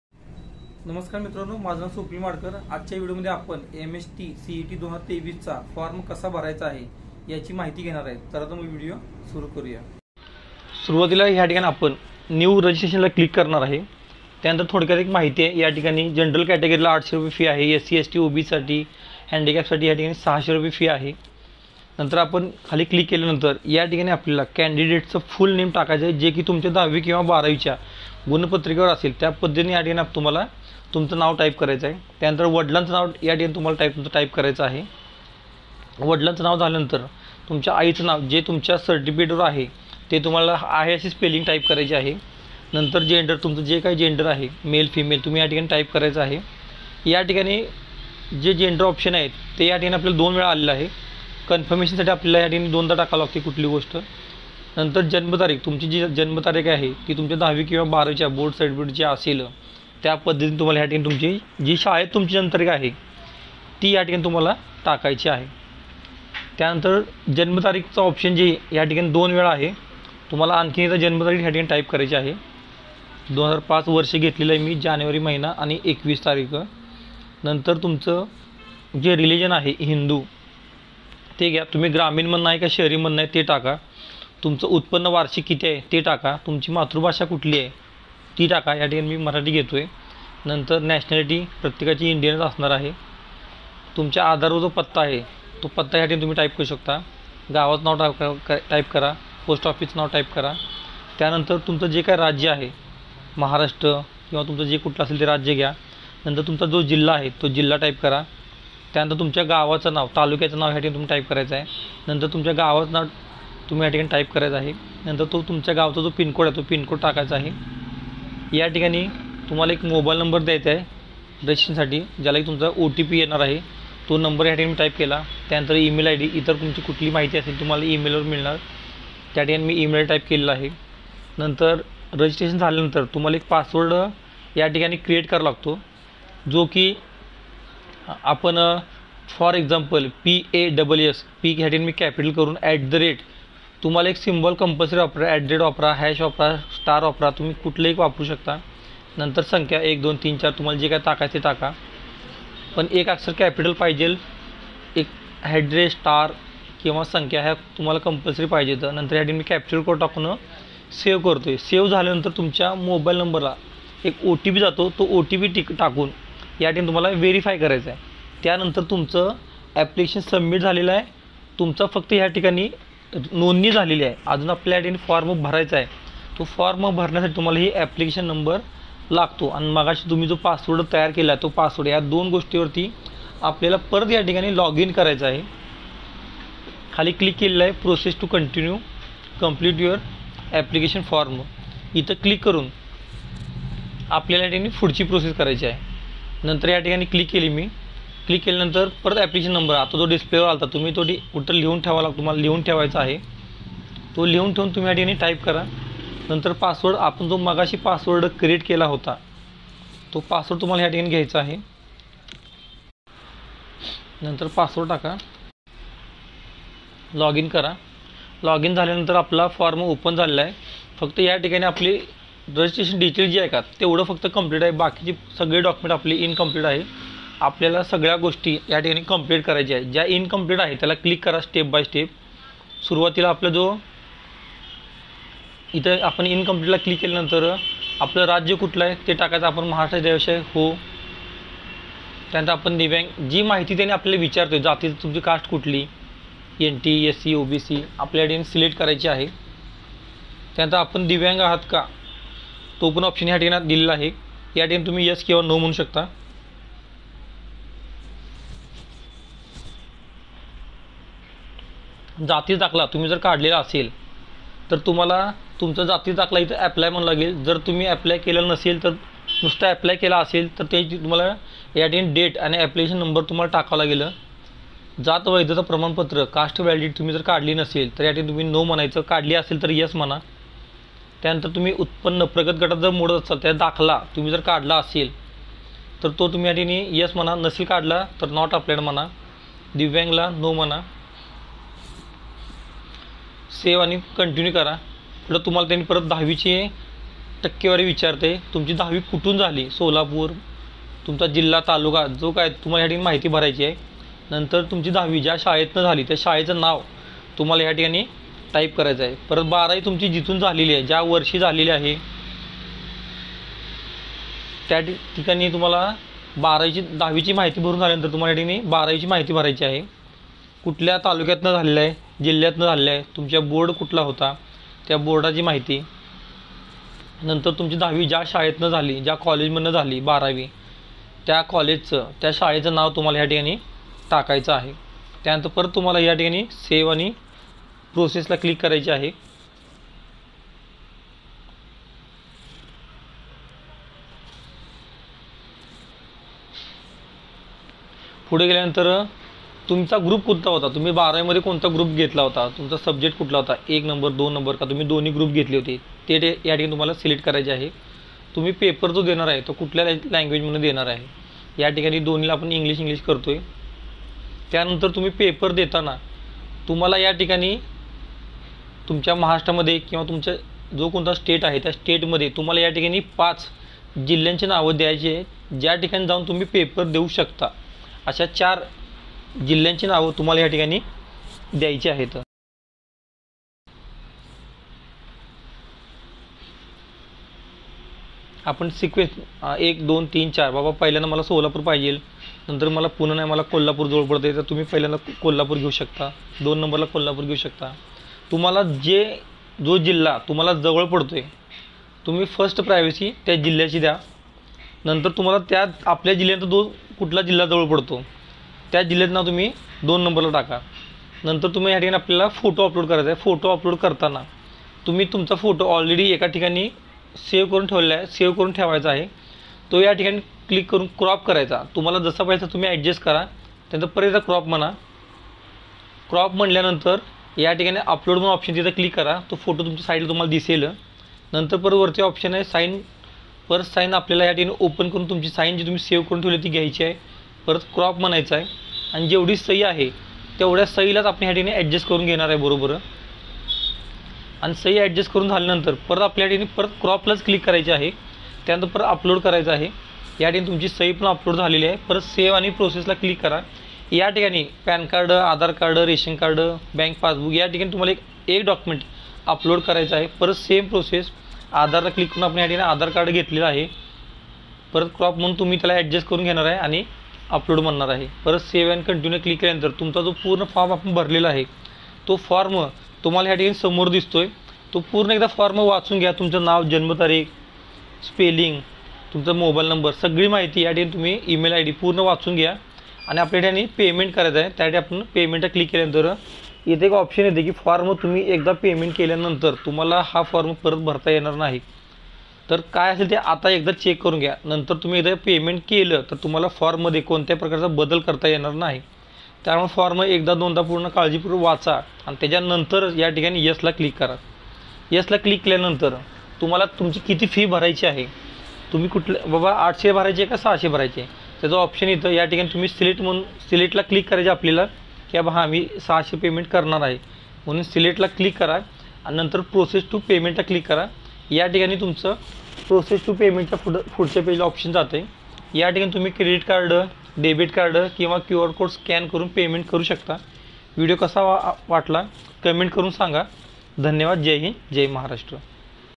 MHT-CET 2023 Registration Started | Full Explanation by swapnil madkar | B.E.
Nasmkar Mitrano.
Maze nav swapnil madkar
aaj apan MHT-CET la registration kase karayache v form kasa bhavayacha he baghanar ahot.